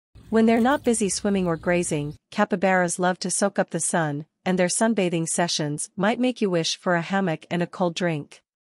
When they're not busy swimming or grazing capybaras love to soak up the sun
When they're not busy swimming or grazing, capybaras love to soak up the sun, and their sunbathing sessions might make you wish for a hammock and a cold drink.